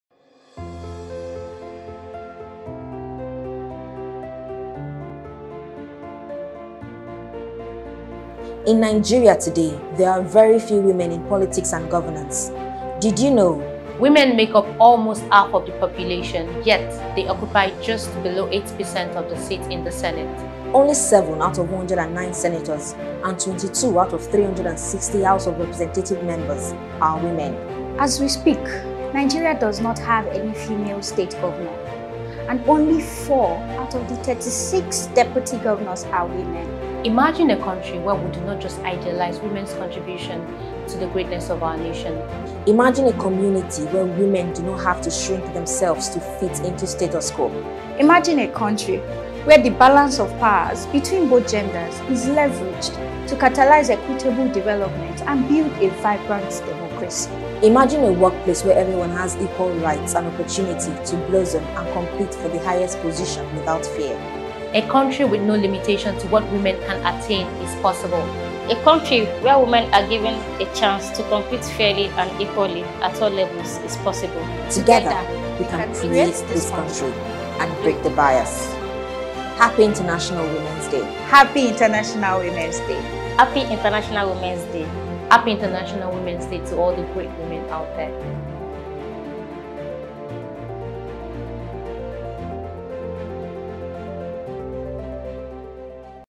Break The Bias
A country where the balance of power between both genders is leveraged to catalyse equitable development.

This is possible.

A beautiful sequence produced by Budgit Communication Teams.